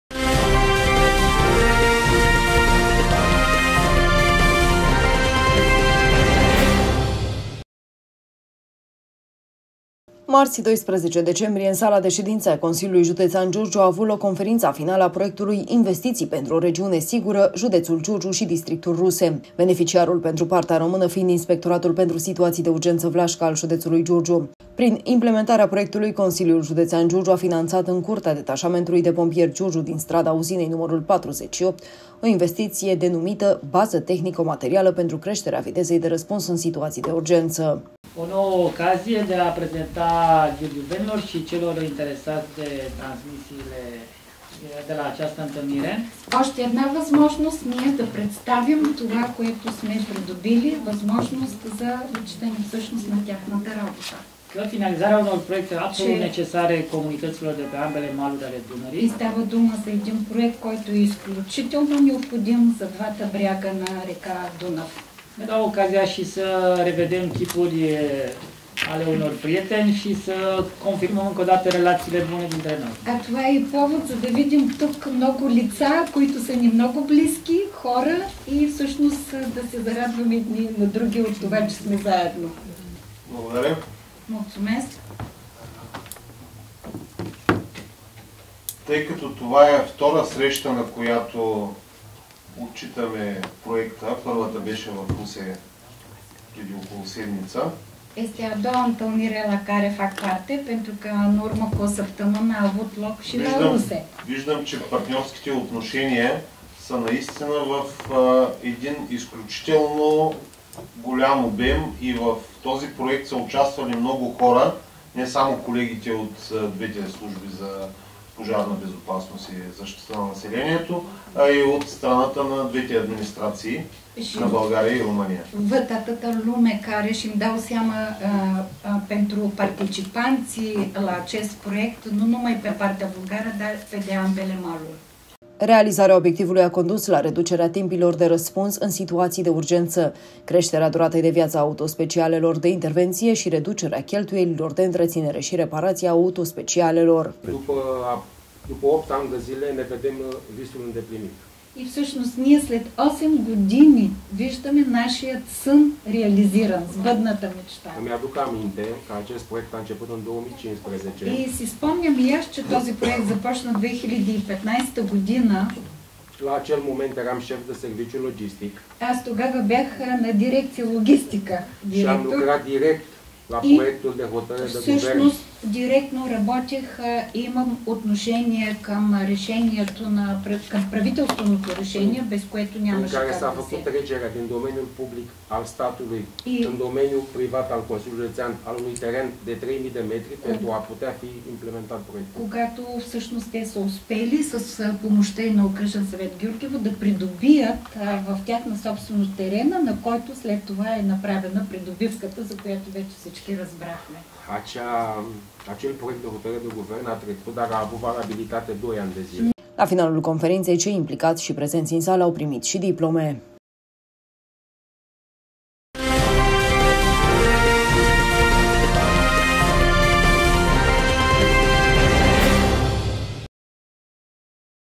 Frăția dintre Giurgiu și Ruse
“Investiții 📃 pentru o regiune sigură: Județul Giurgiu și Districtul Ruse” 🗺️- un proiect dedicat pompierilor 🧑🏻‍🚒 și siguranței cetățenilor 👥